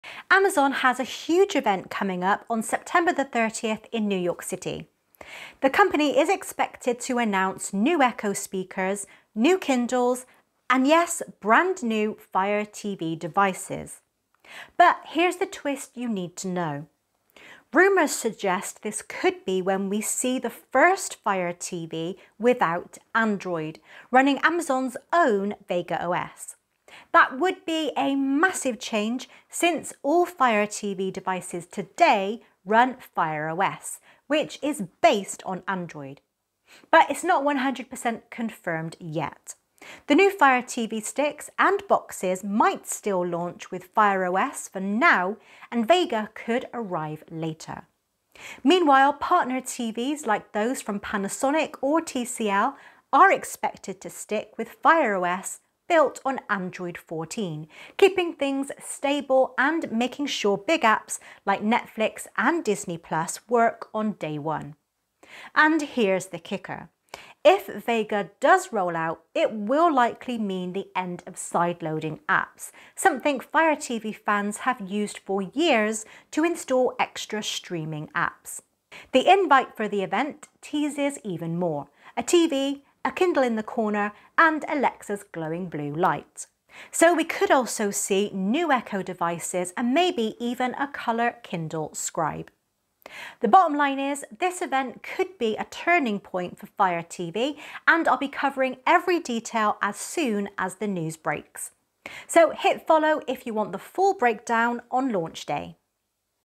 🤯 STOP! Don't Buy a New Fire TV Yet Vega OS vs. Android 14 Explained!
Amazon has confirmed a major event on September 30th in New York City, where new Fire TV devices are expected to be announced — along with Echo smart speakers and new Kindles.
Rumors suggest this could be the launch of the first Fire TV without Android, running Amazon’s new Vega OS. But nothing is confirmed yet — the new sticks and boxes might still ship with Fire OS for now, while partner TVs from brands like Panasonic or TCL are expected to run Fire OS based on Android 14.

If Vega does arrive, it could mean the end of sideloading apps on Fire TV devices, which would be a huge change for streamers.

In this video, I break down:
👉 What Amazon might reveal at the September 30th event
👉 The Vega OS vs Android 14 rumours
👉 What this means for Fire TV users and sideloading